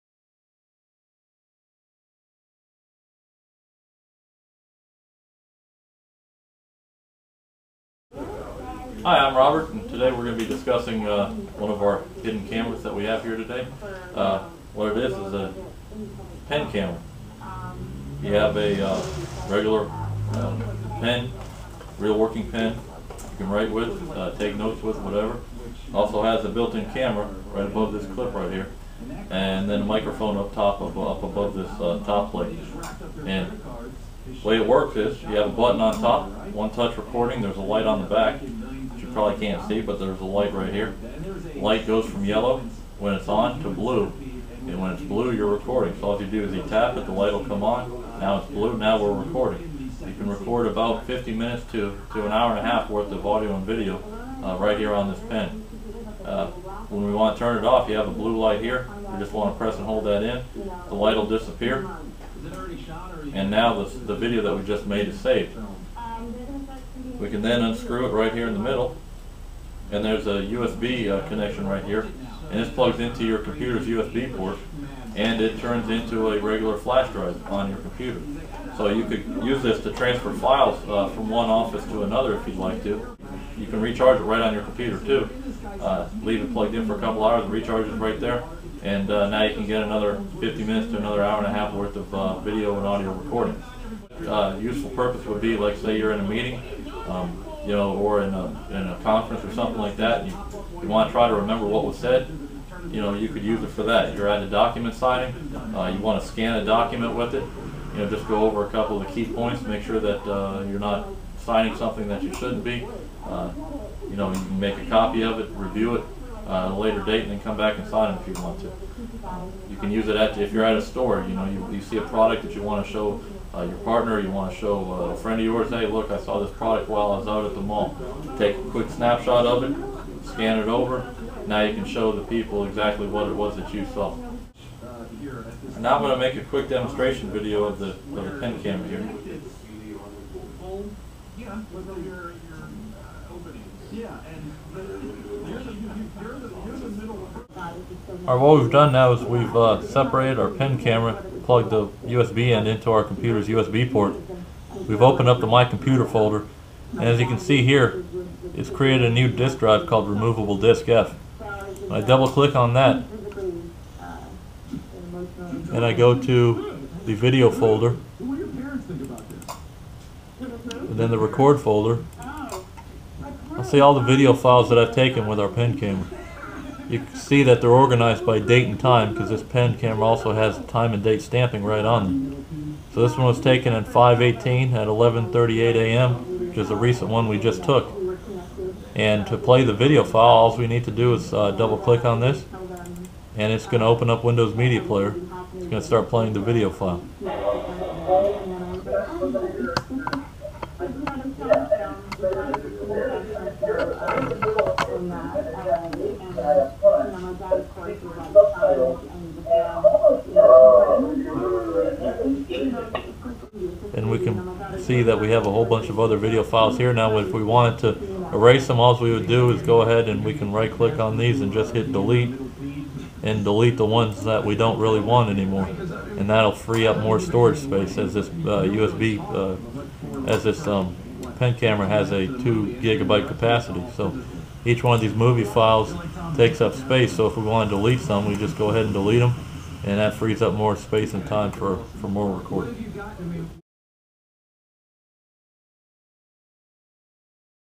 How a Spy Video Camera Pen Works And Records to Flash Drive Then Play Back
Video from our Spy Pen Camera camcorder and  how it works from capturing a video to watching the video on your computer. The new pens also capture still pictures as well. See video from our newest camera at the link above.